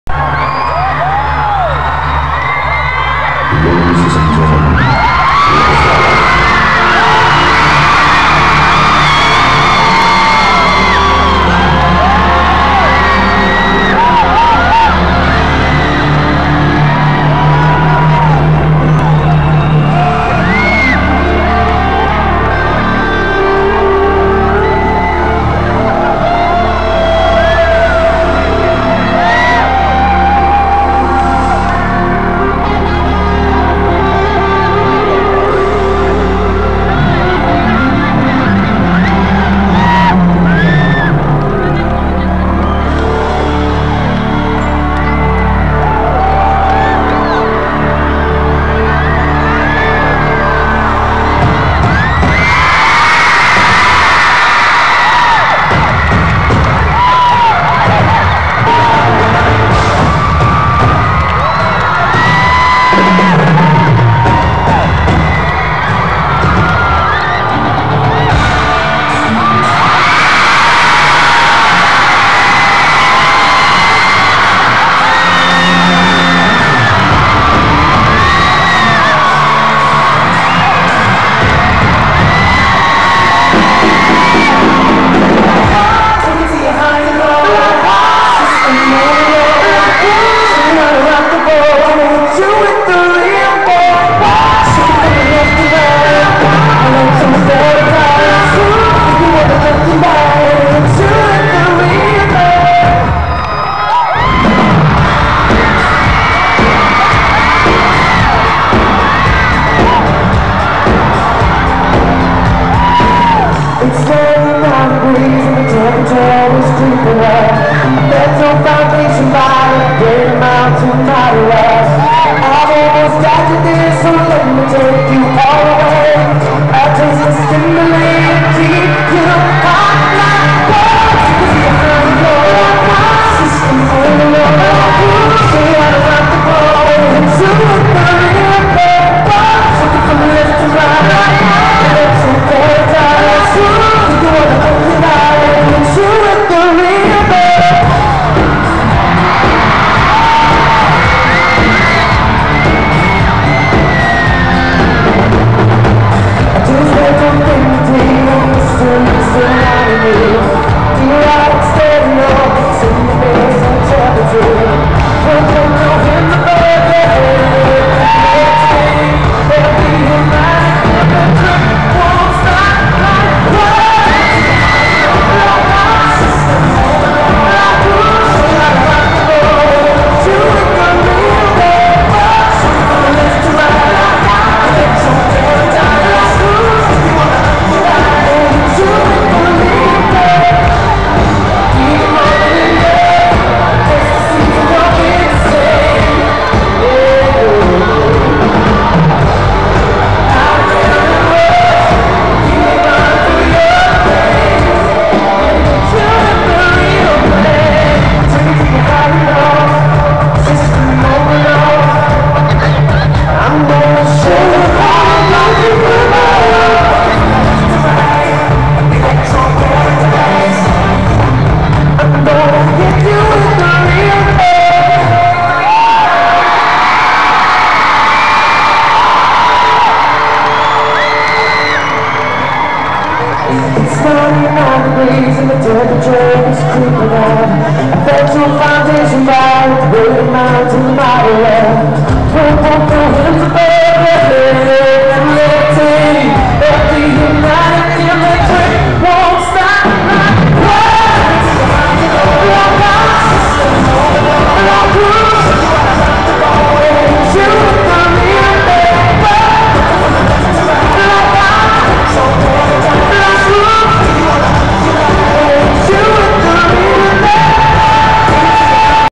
Westlife live in Wellington
Westlife!
it takes a while for you to be able to see them but it gets there